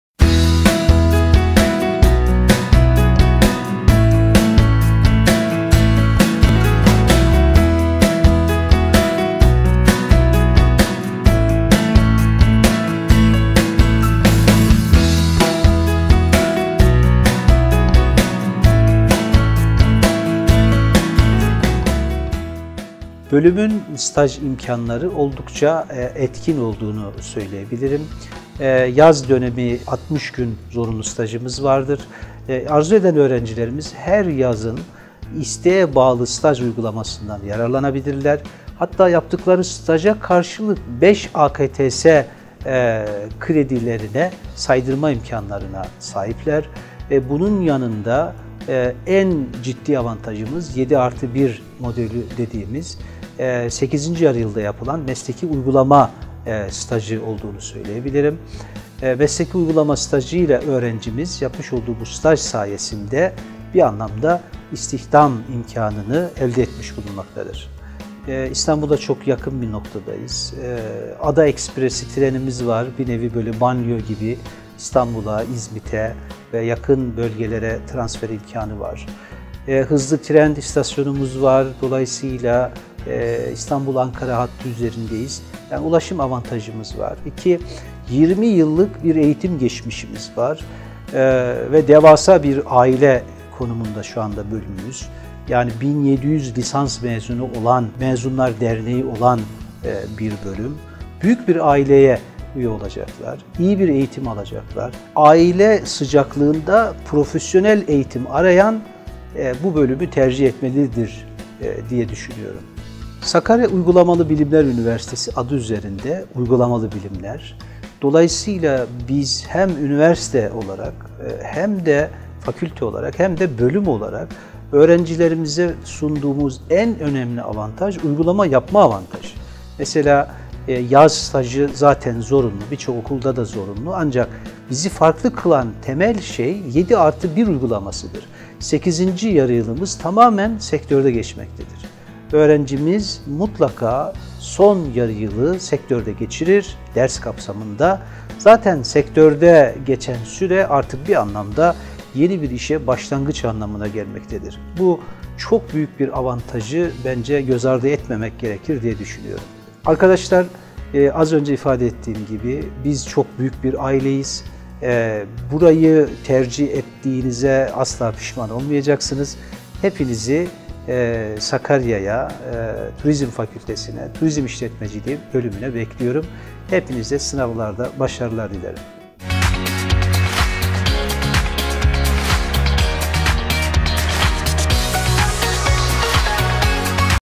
Turizm İşletmeciliği Bölümü - 2
Turizm Fakültesi Turizm İşletmeciliği Bölümü Başkanı Prof. Dr. Orhan Batman, bölümü anlatıyor.